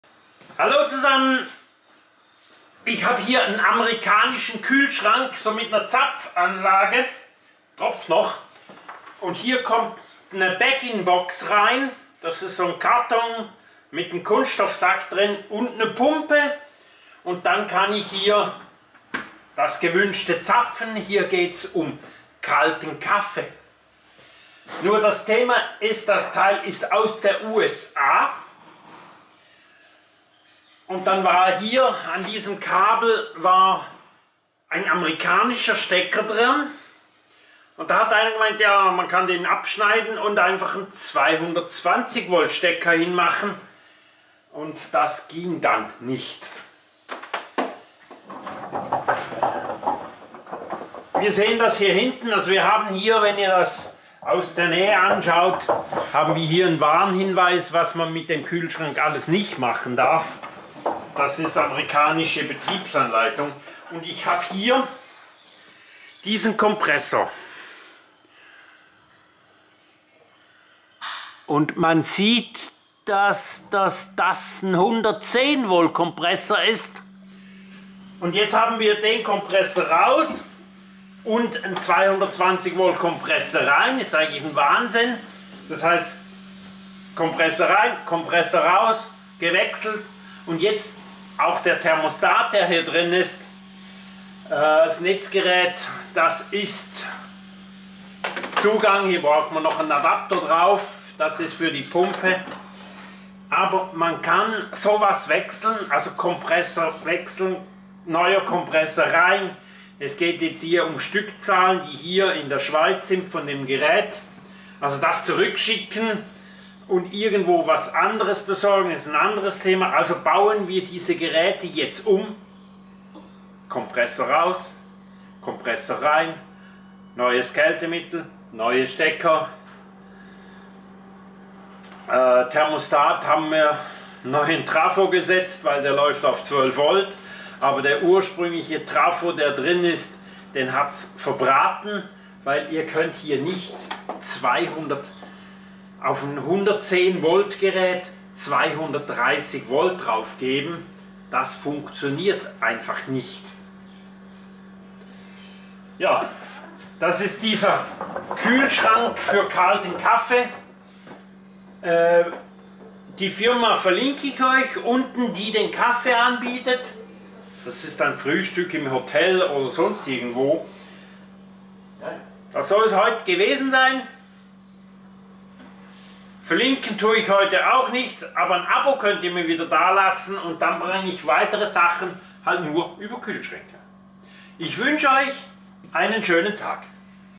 Kühlschrankumbau von 110 auf 230 Volt Bericht Erfahrung Kompressor ersetzt Bevis.ch kalter Kaffee
USA Kühlschrank auf 230 Volt umgebaut